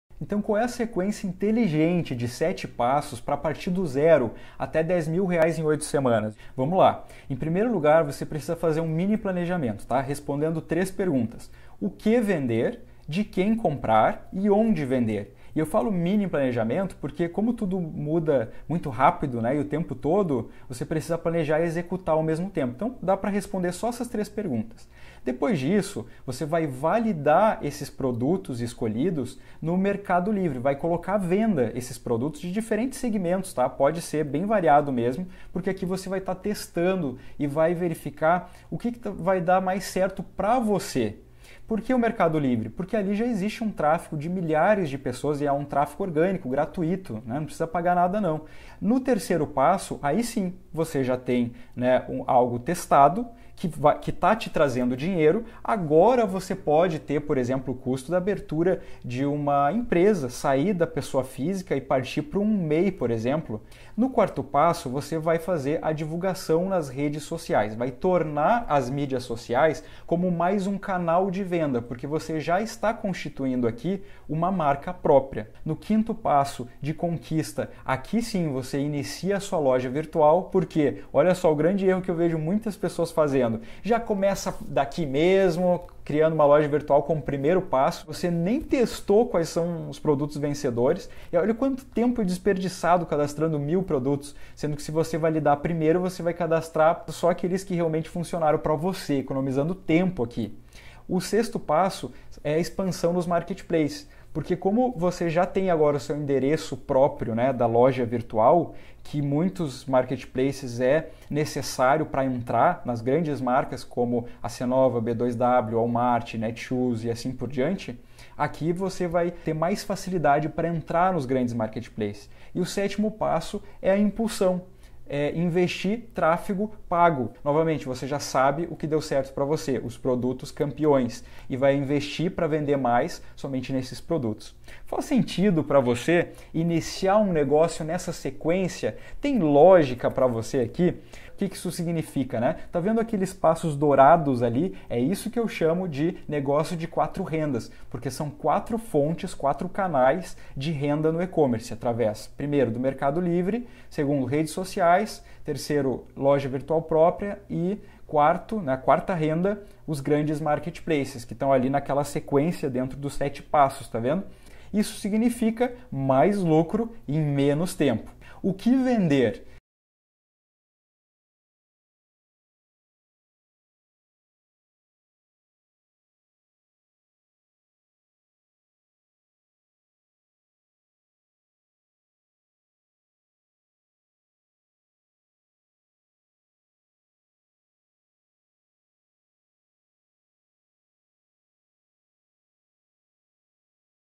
Como Montar um E commerce de Sucesso em 7 Passos Muquiranos
Nesse vídeo vou te ensinar como vender no mercado livre sem estoque e sem precisar gastar um centavo.